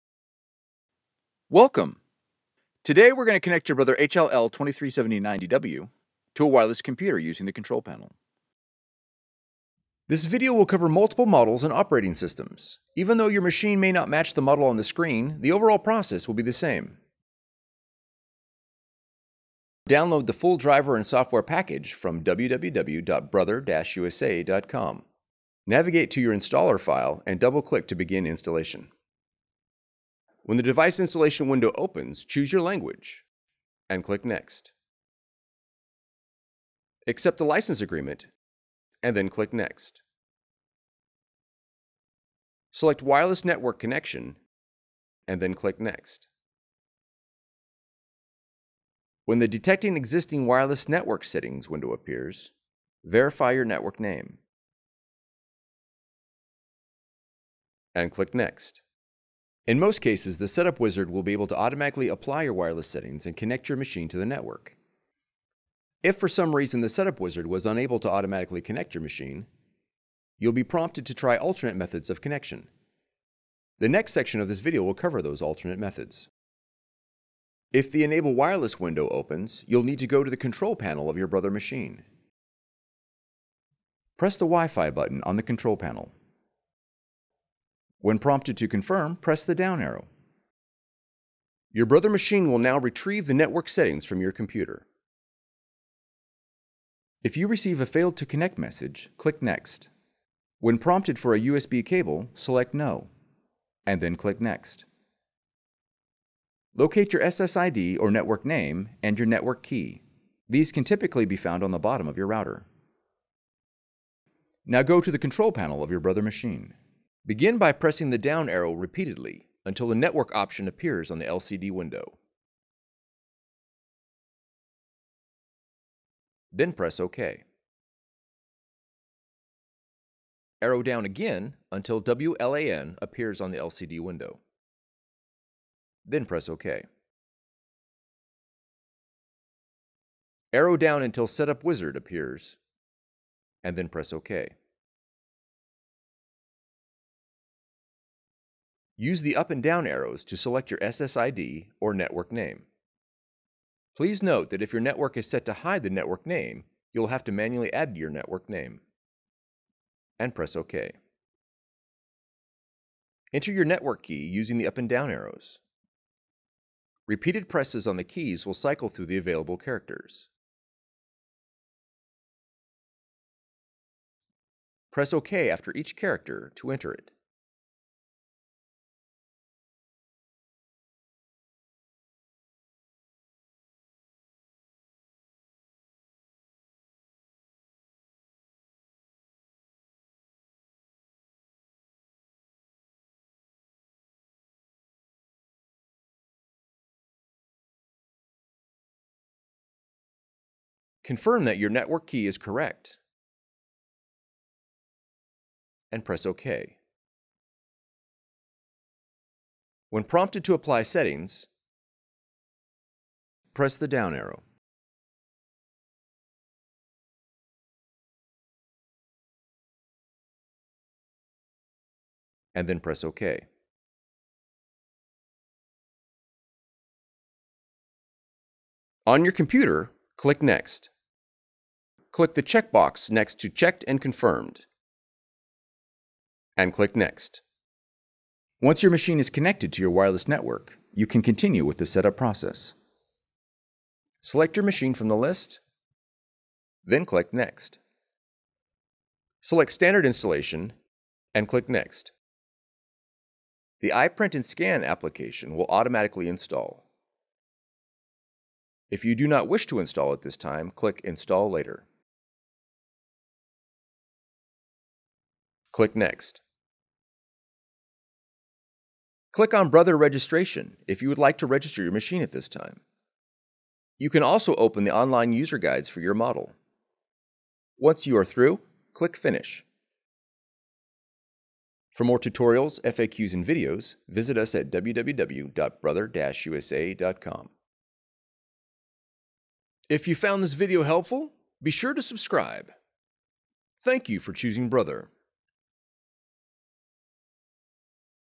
Connect HLL2379DW to a wireless computer - Windows
Learn how to connect your Brother HL-L2379DW laser printer to a wireless computer using Windows in this video tutorial. This method will use your machine’s control panel to establish the wireless connection.

Models covered:
Brother HLL2379DW

Quick Links / Table of Contents:
Intro 00:00
Models Covered 00:09
Insert or Download the software 00:19
Running the Installer software 00:32
          Alternate connection methods 01:00
          Enable Wireless window 01:18
          Failed to Connect window 01:38
          Locate SSID and Network Key 01:47
          Machine control panel 01:56
Continue setup with the Installer 3:45
iPrint&Scan install 4:15
User’s Guide and Registration 04:30

Transcript:
Welcome. Today we're going to connect your Brother HL-L2379DW to a wireless computer using the setup wizard

This video will cover multiple models and operating systems even though your machine may not match the model on the screen the overall process will be the same. (Video covers HL-L2350DW and HL-L2370DWXL wifi setup too)

Insert the installation disc or download the full driver and software package

Navigate to your installer file and double-click to begin installation. When the device installation window opens choose your language and click "Next."

Select wireless network connection and click "Next."

When the “Detecting existing wireless network settings” window appears verify your Network Name and click “Next”

In most cases the setup wizard will be able to automatically apply your wireless settings and connect your machine to the network.

If for some reason the setup wizard was unable to automatically connect your machine, you will be prompted to try alternate methods of connection. The next section of this video will cover those alternate methods.

If the Enable Wireless window opens you will need to go to the control panel of your Brother machine

Press the Wi FI button on the control panel. When prompted to confirm, press the down arrow

Your Brother machine will now retrieve the network settings from your computer

If you receive a Failed to Connect message click “next.”

When prompted for a USB cable select “No” and then click next

Locate your SSID or Network name and your Network Key. These can typically be found on the bottom of your router.

Now go to the control panel of your Brother machine

Begin by pressing the down arrow repeatedly until the “Network” option appears on the LCD window. Then press “OK”

Arrow down again until “WLAN” appears on the LCD window. Then press “OK

Arrow down until “Setup Wizard” appears then press “OK”

Use the up and down arrows to select your SSID or Network Name and press “OK” Please note that if your network is set to hide the network name you will have to manually add your network name.

Enter your Network Key, using the up and down arrows. Repeated presses on the keys will cycle through the available characters. Press “OK” after each character to enter it.

When prompted to apply settings, press the down arrow

Once “Connected” appears on the LCD, press “OK”

On your computer click “Next”

Click the checkbox next to “Checked and confirmed” and click “Next”

Once your machine is connected to your wireless network, you can continue with the setup process

The iPrint & Scan application will automatically install. If you don’t wish to install at this time click “Install Later”

Click "Next"

Click on Brother registration if you would like to register your machine at this time. You can also open the online user guides for you model. Once you are through click "Finish"

Customers also searched:
Brother HL L2379DW Windows wireless
Brother HL L2379DW Windows setup wizard